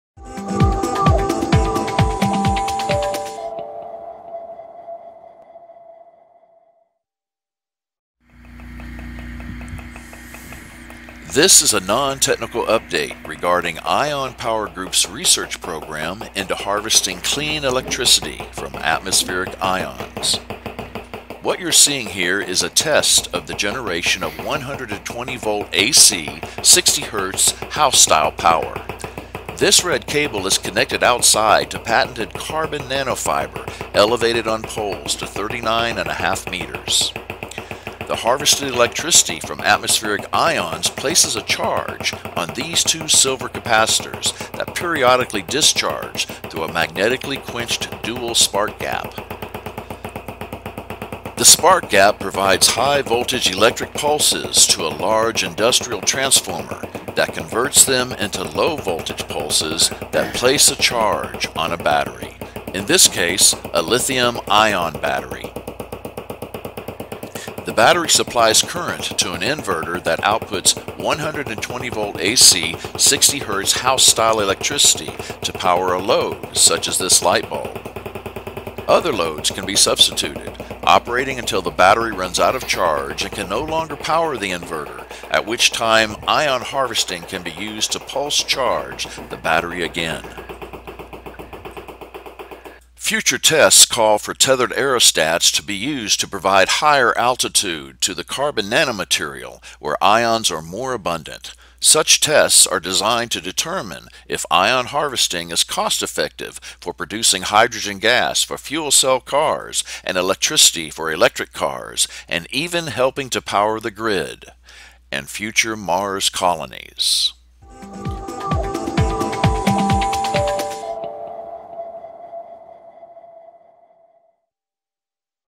IonPowerGroup com Update 11 12 2018
Ion Power Group is an energy research and development company with patents approved in 33 countries on a revolutionary method of utilizing patented carbon nanomaterial to harvest atmospheric ions to generate clean electricity day and night.